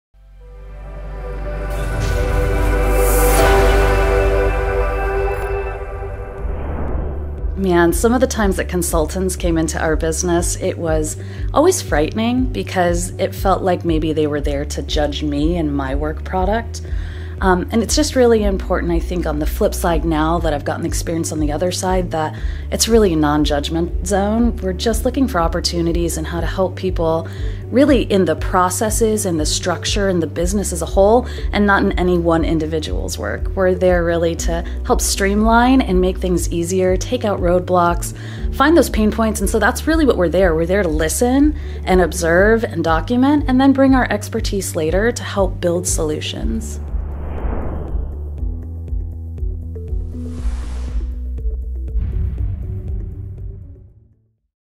INSIDE INSITE CIARA COOPER What Consultants Do 001
An in-depth report on what an Insite consultant does when they visit a client site.